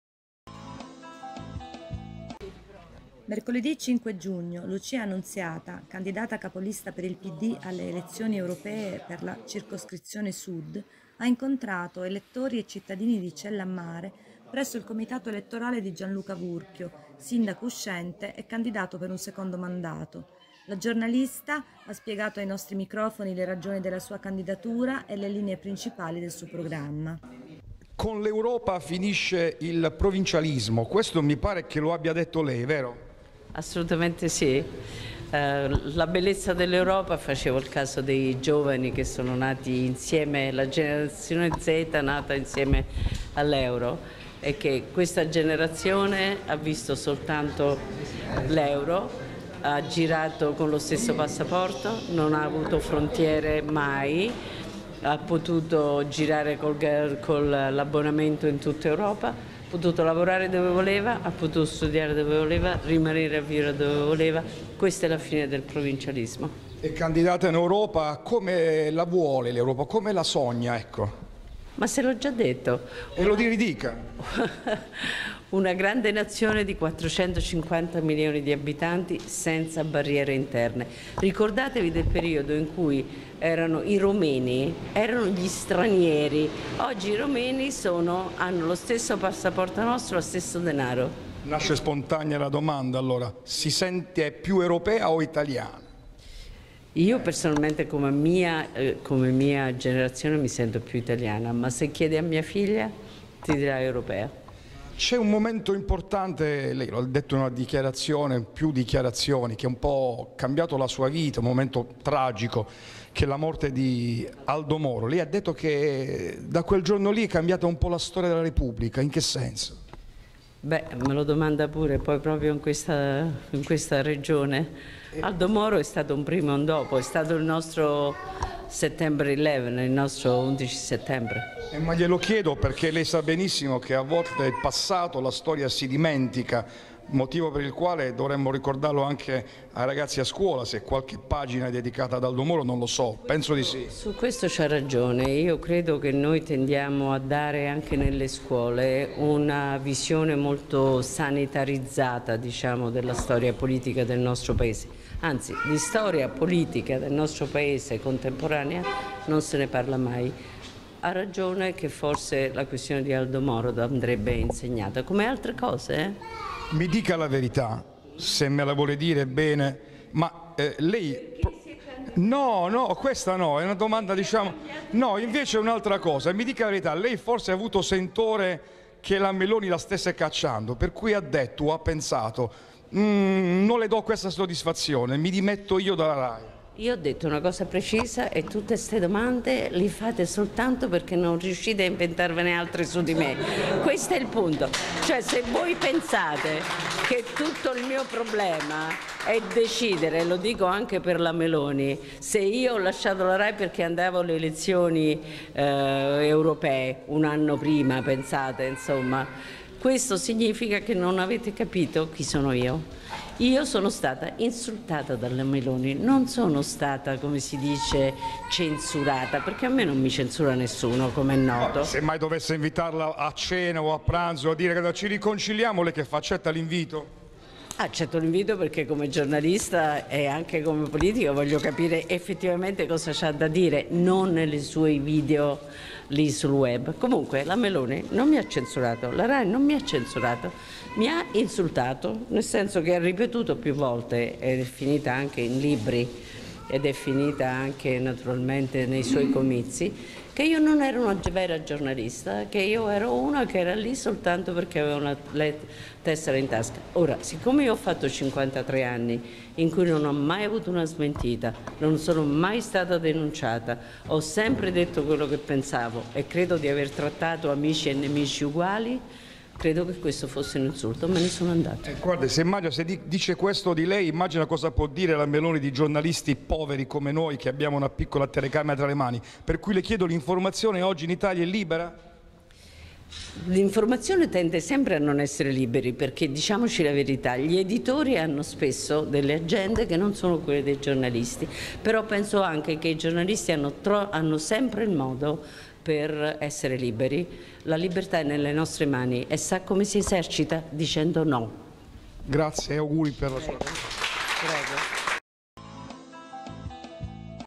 Lucia Annunziata a Cellamare: ospite d'eccezione al Comitato Elettorale di Gianluca Vurchio
Lucia Annunziata a Cellamare: ospite d'eccezione al Comitato Elettorale di Gianluca Vurchio  (  5 giugno 2024)

intervista di Antonio Carbonara